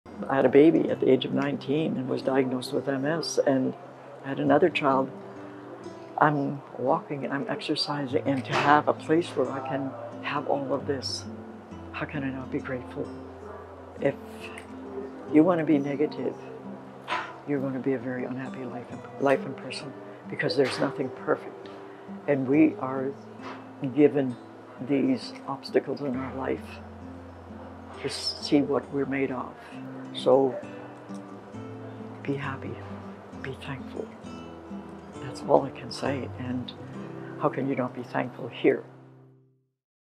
Mary, Be Happy Be Thankful
Testimonial of residents who stay at The Melody